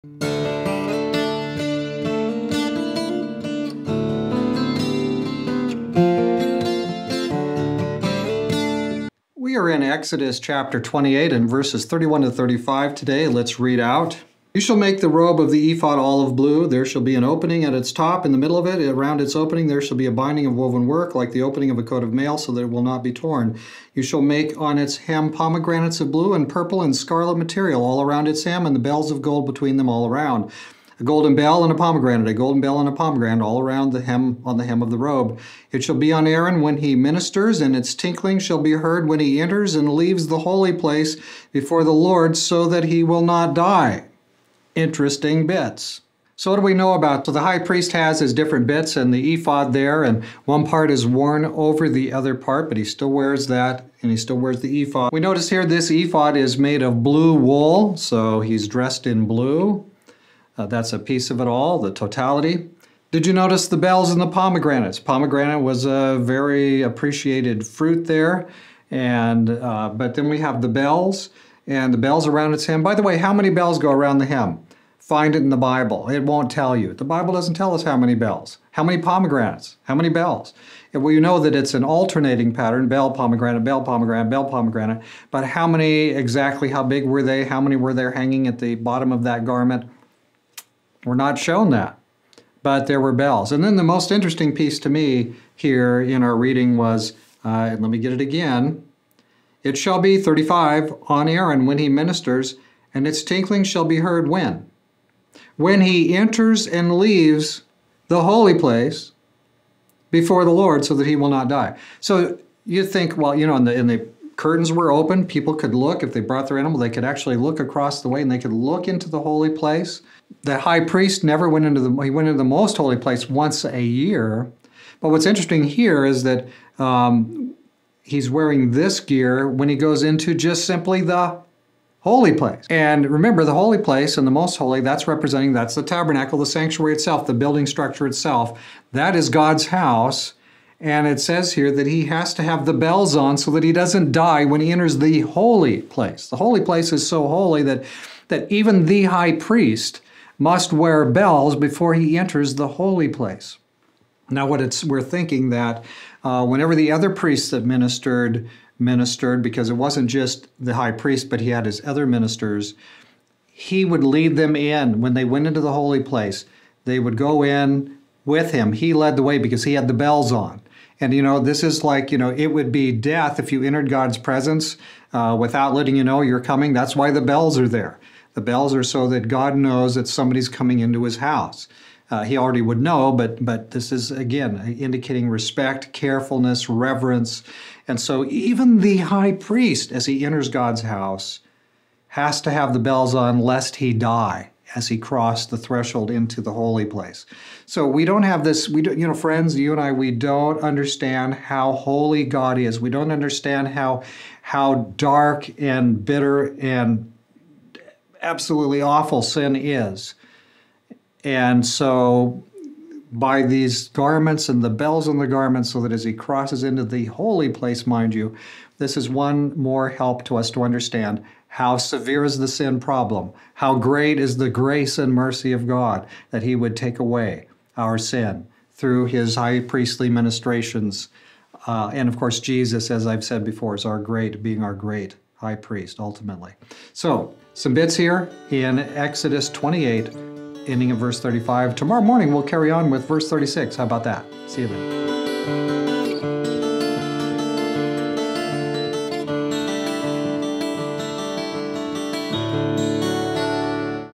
Exodus 28:31-35 Bells on or Die
Even the high priest, even when entering merely the holy place, must sound his entrance with bells on his robe or else die. God is holy.